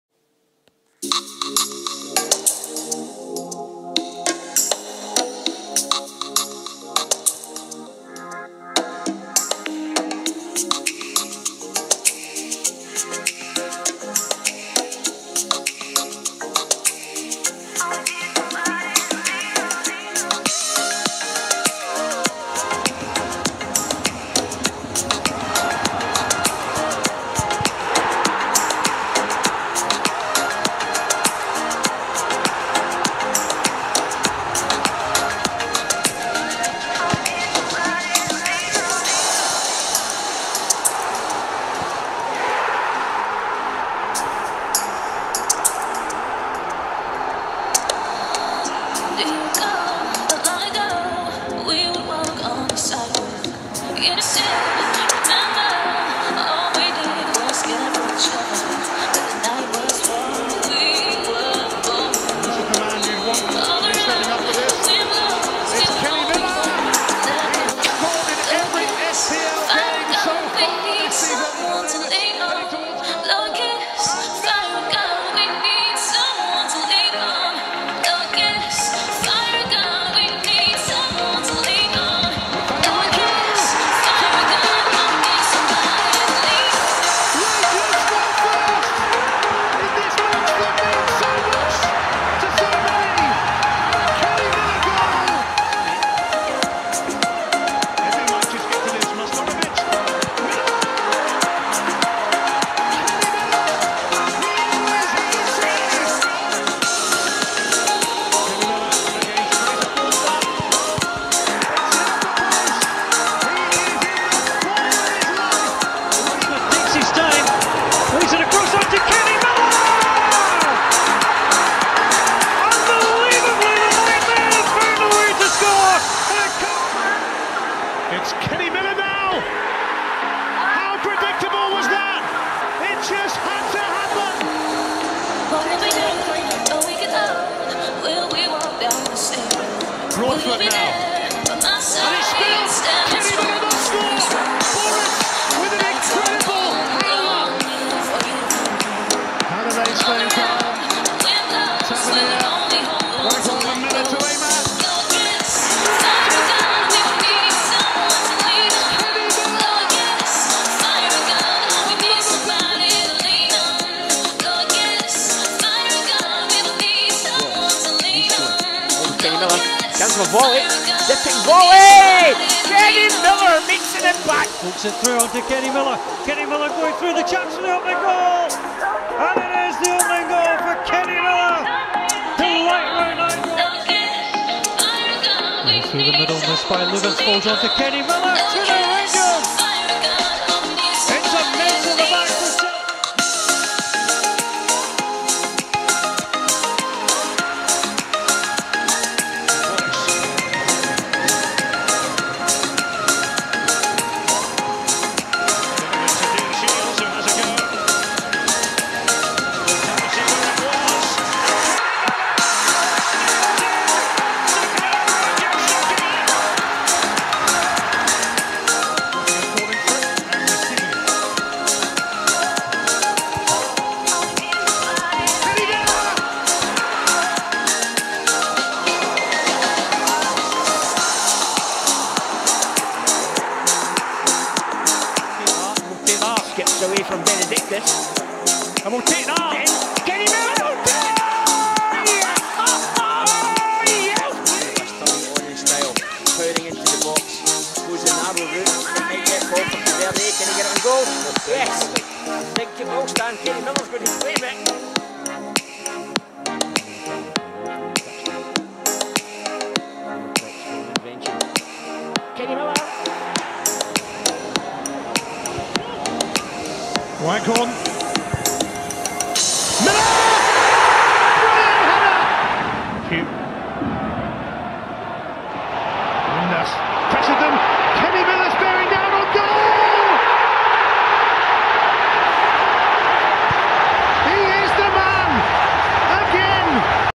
Kenny Miller Rangers Goals
A few of my favourite goals from King Kenny.